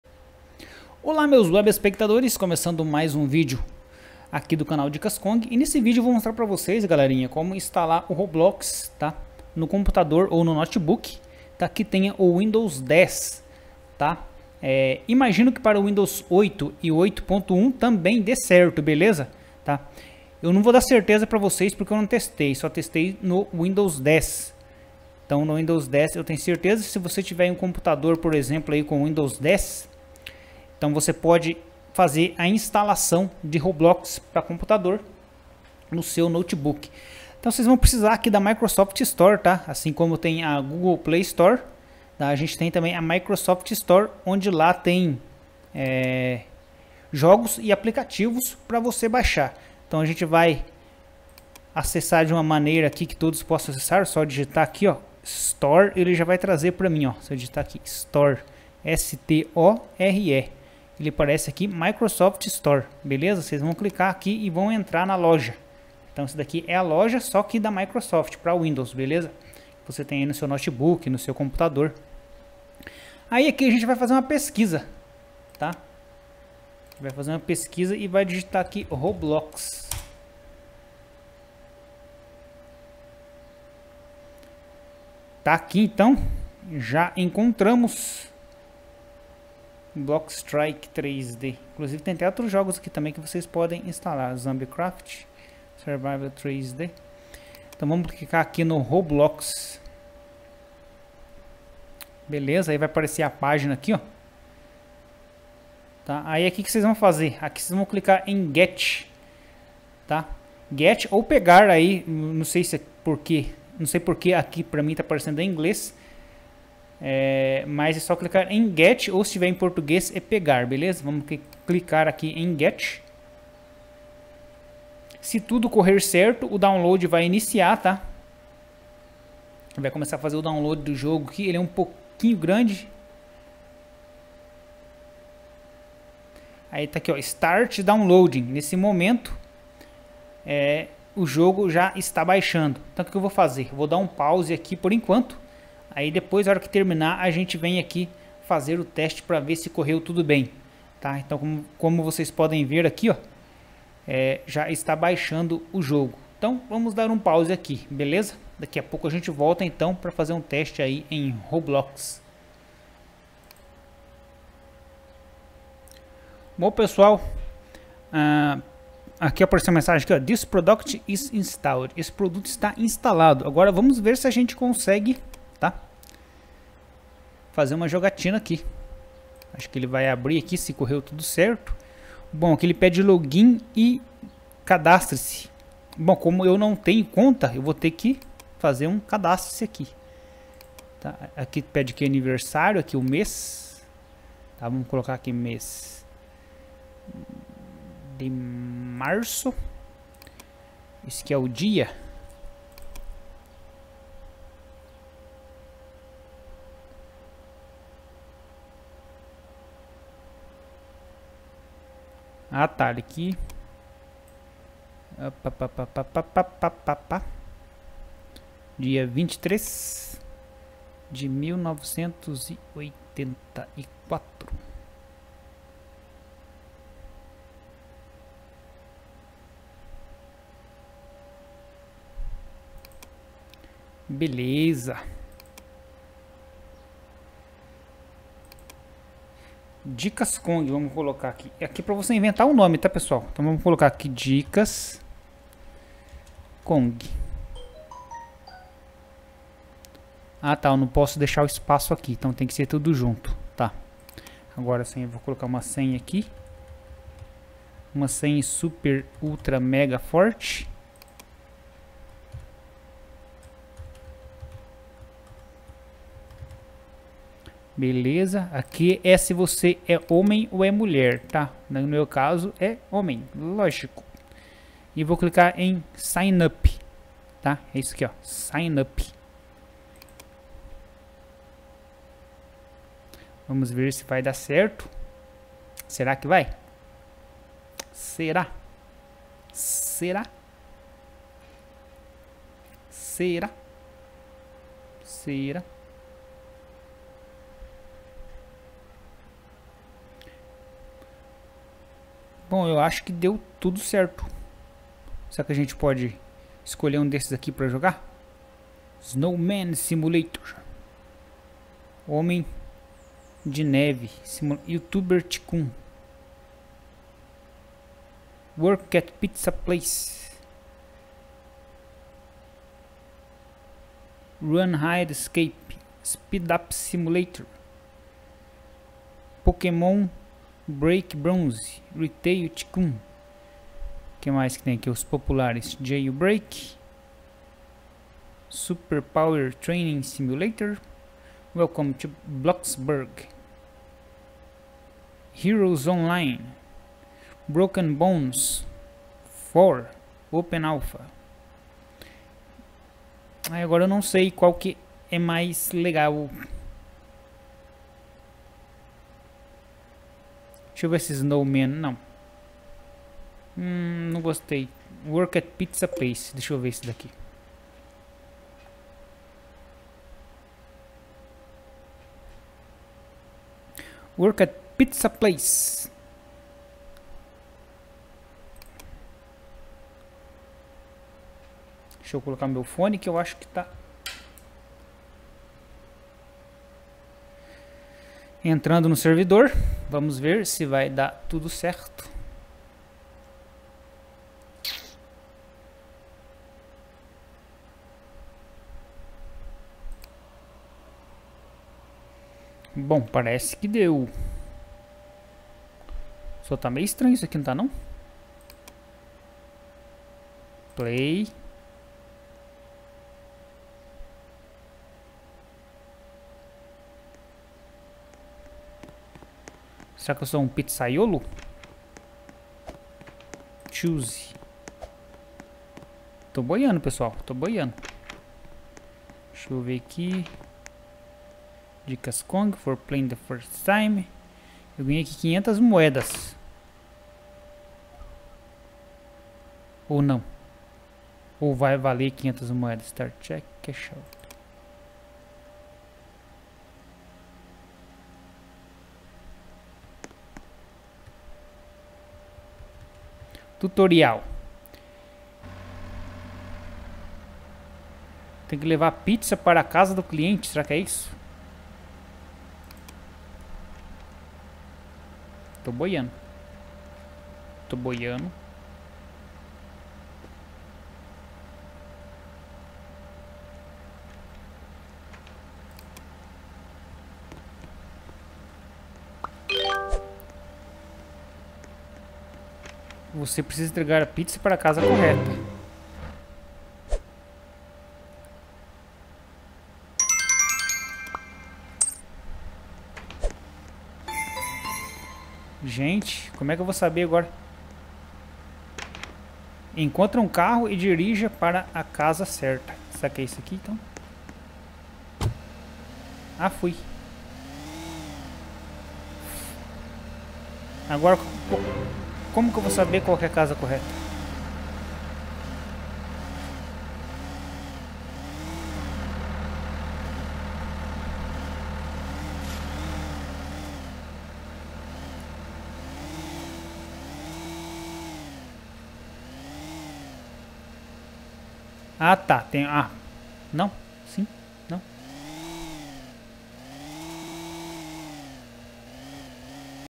Roblox | Como baixar e instalar no PC ou Notebook | Windows 10
Neste vídeo vou mostrar como instalar o jogo Roblox em um PC (Computador com Windows 10) A instalação é tranquila e rápida visto que o jogo é bem leve o pacote de instalação. Espero que este vídeo te ajude de alguma forma.